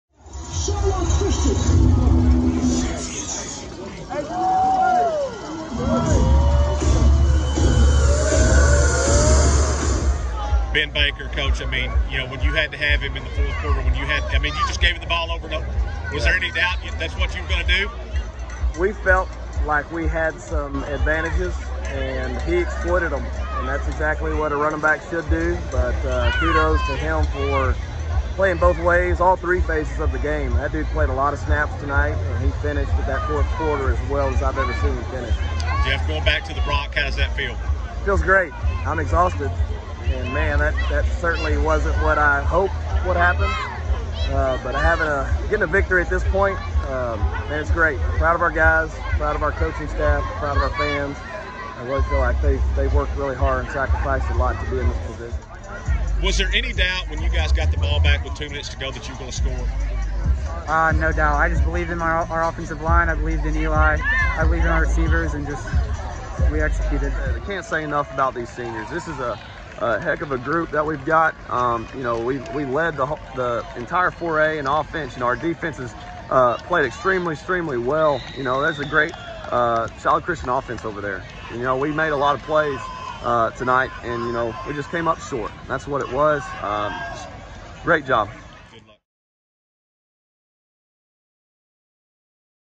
Arkadelphia vs Shiloh Christian
Interview with the head coaches from Arkadelphia and Shiloh Christian after their game Friday Dec. 17, 2021.
Sign up for our free Prep Sports newsletter